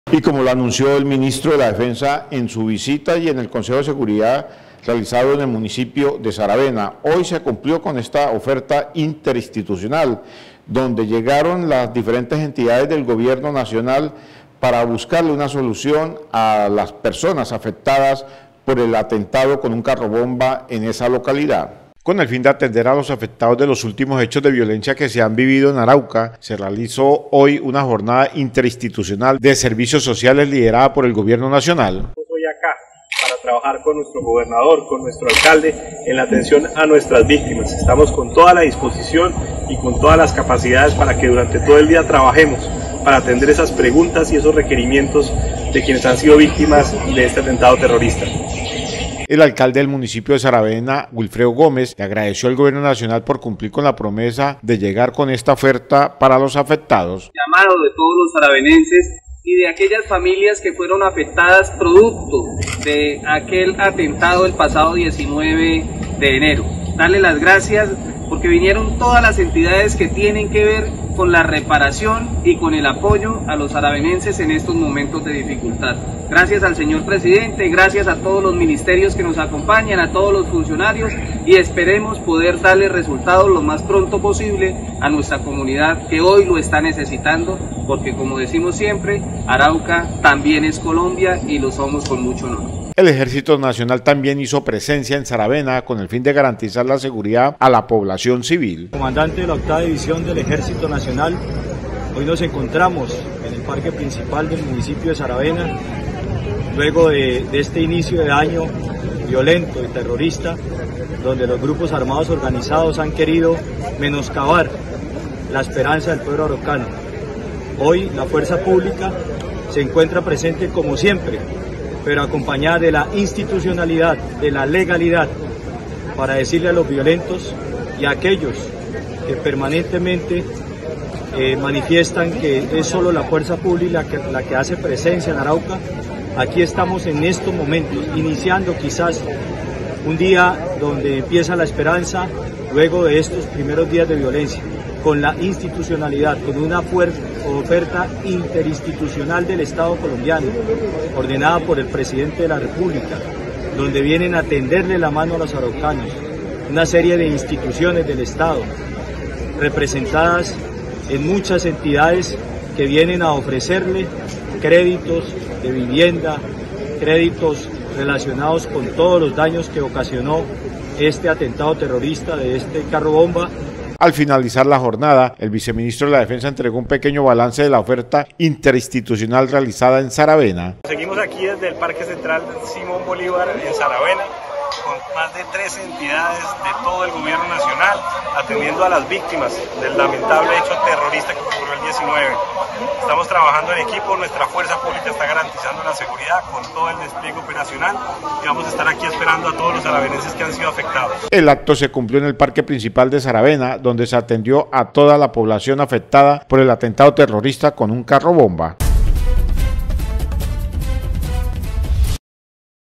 Gobierno realizó jornada Interinstitucional a los afectados por el Atentado Terrorista en Saravena
Gobierno Nacional realizó jornada Interinstitucional para los afectados por el Atentado Terrorista en Saravena